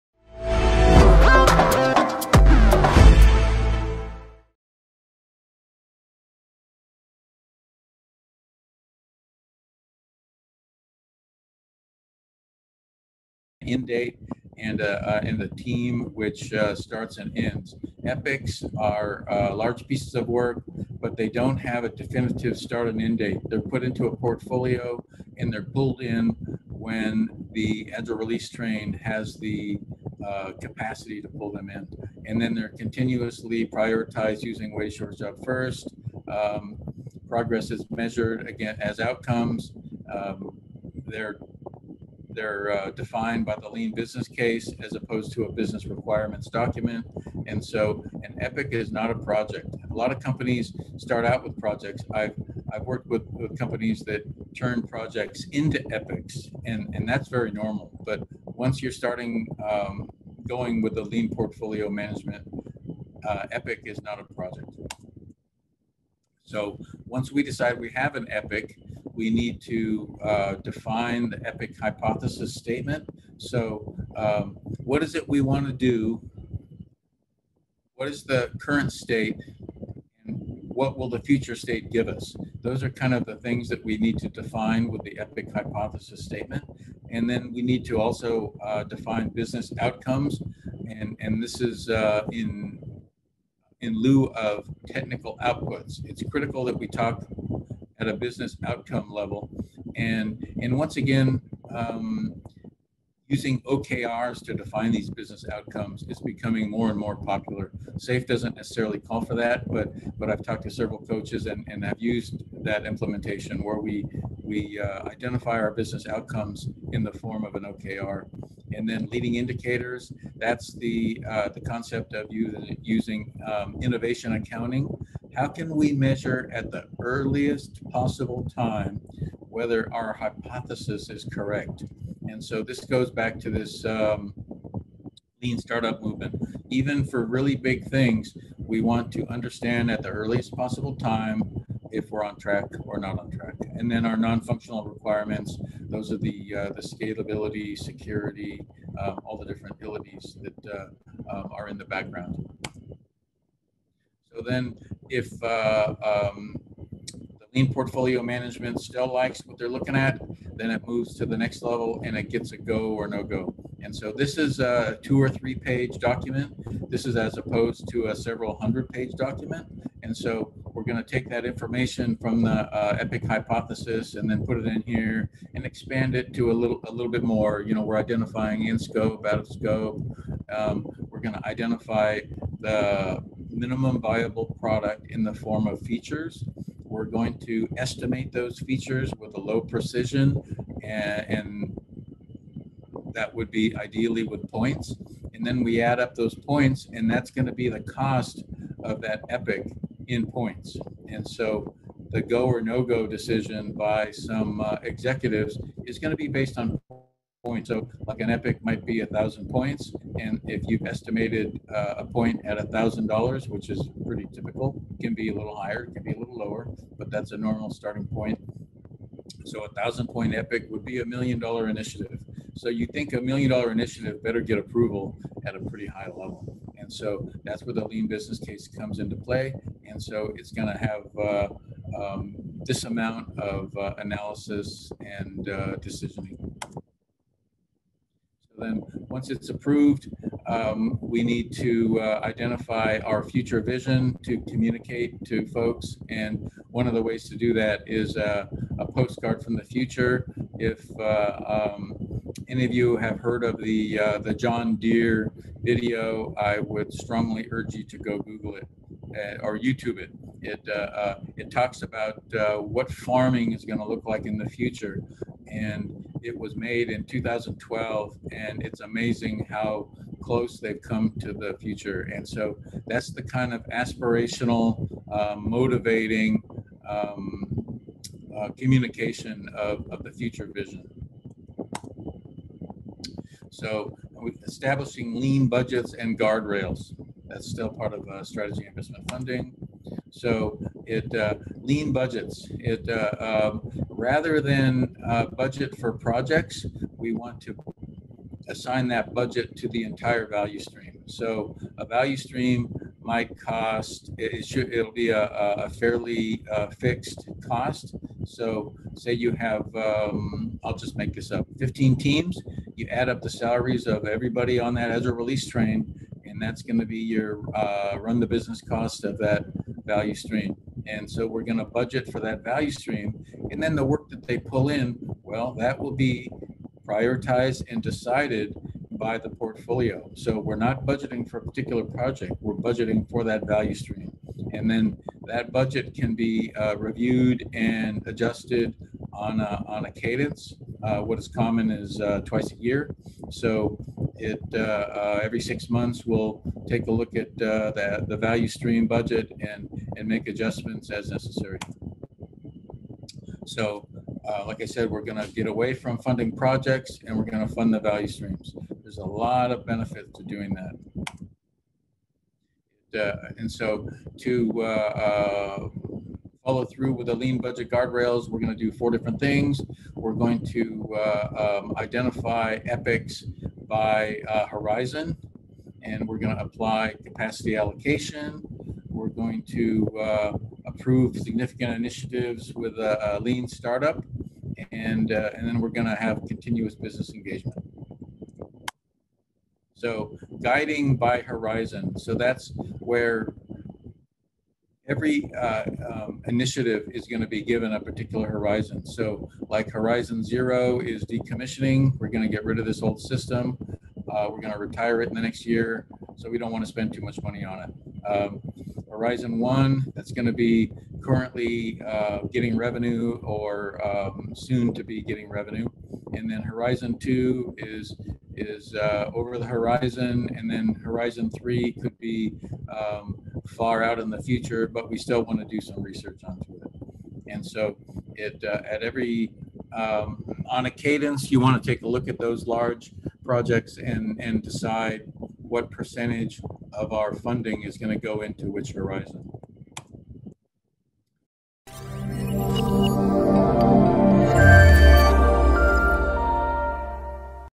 Epics- from The Scaled Agile Framework Lean Portfolio Management Webinar
Guest Speaker: Allen Ward

Nov 4, 2020 @ExpertAgileClub